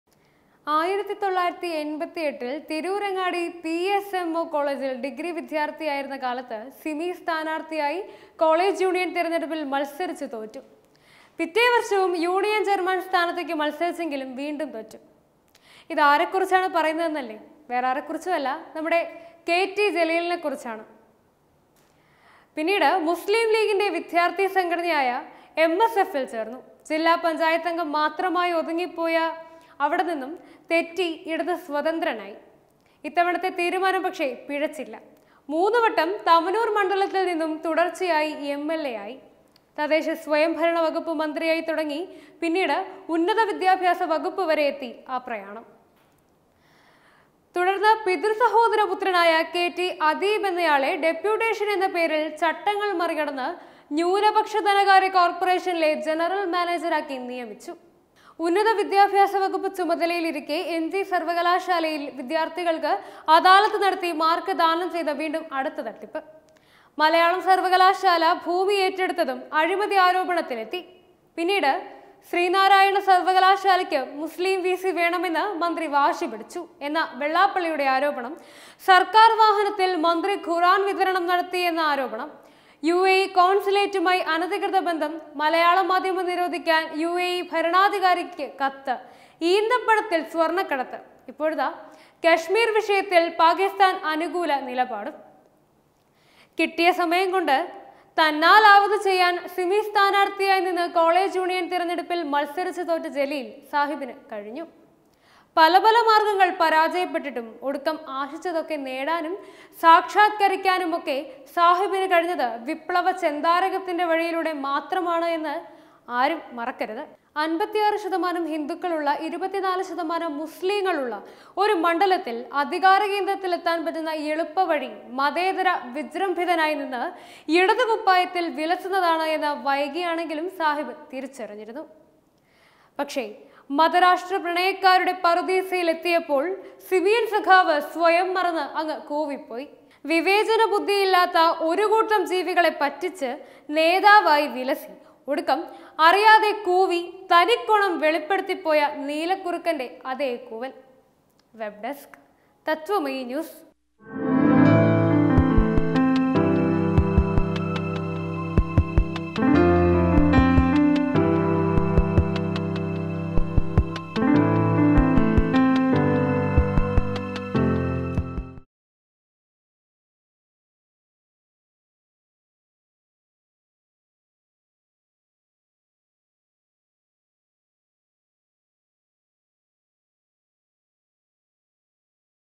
ഇത് 'മ്മടെ' സിമി നേതാവ് ജലീലിന്റെ ചരിത്രം വന്ന വഴി കണ്ടാൽ നിങ്ങൾ ഞെട്ടും | KT Jaleel
ചട്ടങ്ങൾ മറികടന്ന് ന്യൂനപക്ഷ ധനകാര്യ കോർപറേഷനിലെ ആദ്യ പിൻവാതിൽ നിയമനം നടത്തി...? അധികാര കേന്ദ്രത്തിൽ എളുപ്പത്തിൽ എത്താൻ മതേതര വിജ്രംഭിതനായി നിന്ന് ഇടത് കുപ്പായത്തിൽ കയറി ? ഇത് ജലീലിന്റെ ചരിത്രം | KT Jaleel